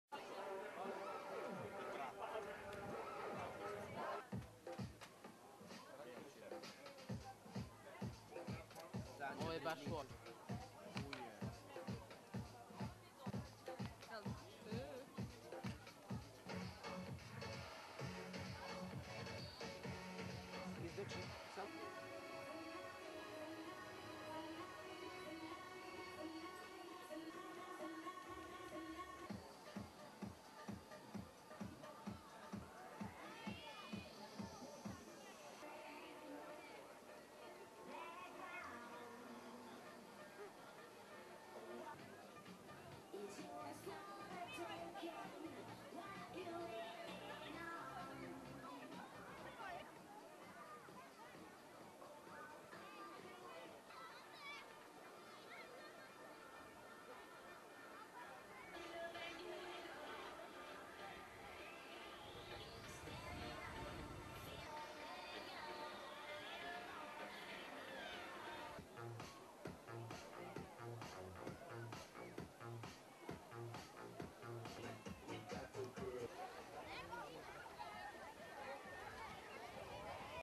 saznaj i izrazi se na bazenu
Aktivisti Medijskog istrazivackog centra dele flajere, na bazenu Cair, koji pozivaju mlade da se ukljuce u akciju "Saznaj i izrazi se".